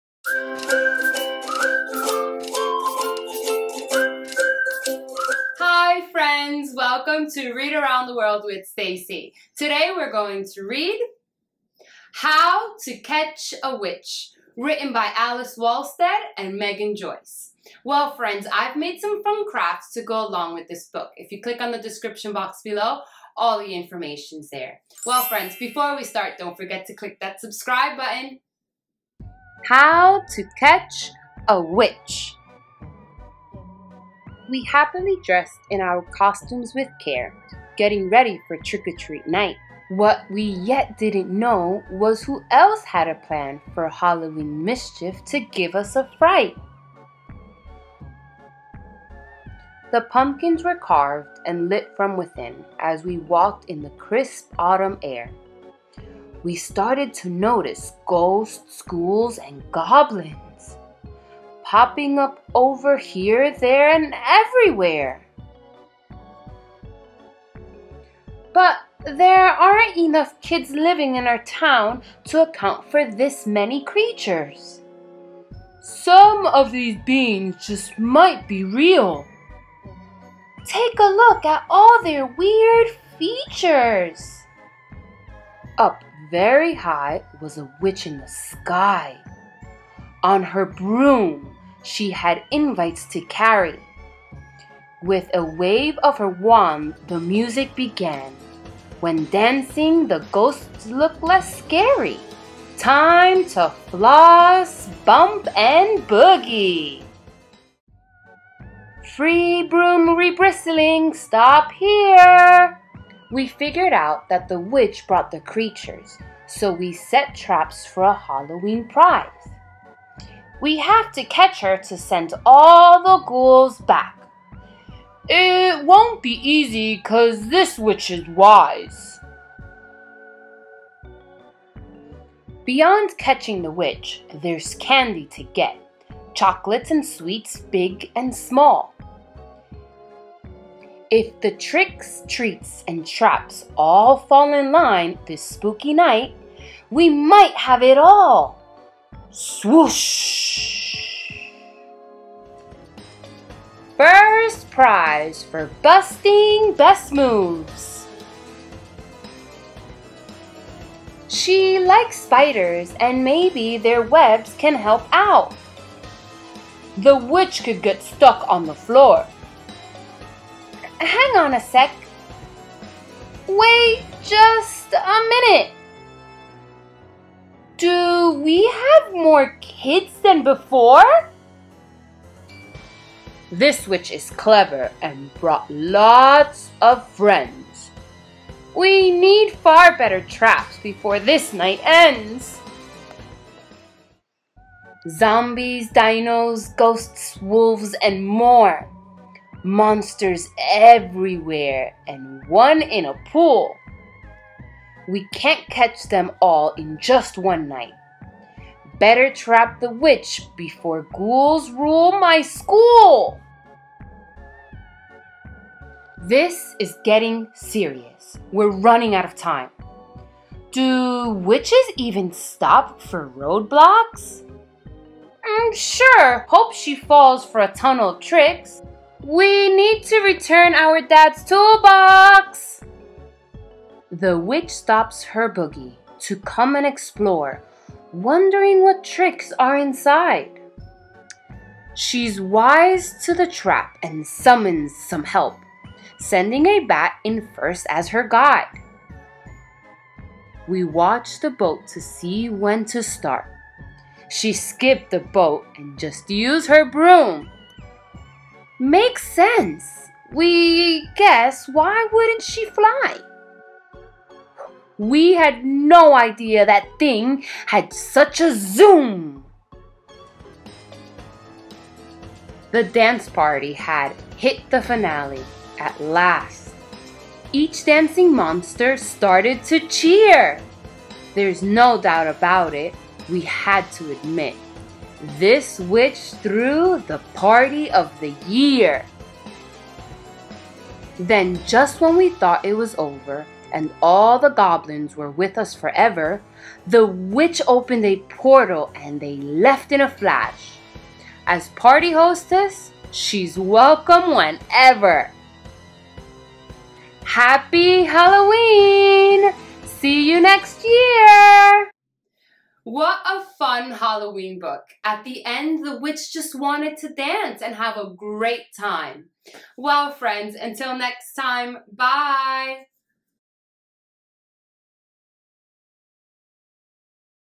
🧙‍♀️How To Catch A Witch Halloween Read Aloud Story for Kids
Activities come with this read-aloud so go check it out!

Craft: How to Catch a Witch Halloween Craft!

How to Catch a Witch  by Alice Walstead (Author), Megan Joyce (Illustrator)
Publisher: Sourcebooks

It's Halloween night and something doesn't seem quite right... When the Catch Club Kids head out to trick-or-treat, there are ghosts, goblins, and ghouls everywhere! Our heroes discover a witch on a broom has opened a magical portal that let out spooky creatures all over the neighborhood. Follow along in this frightfully fun story as they set zany traps for the witch to close the portal and save Halloween before all the candy is gone!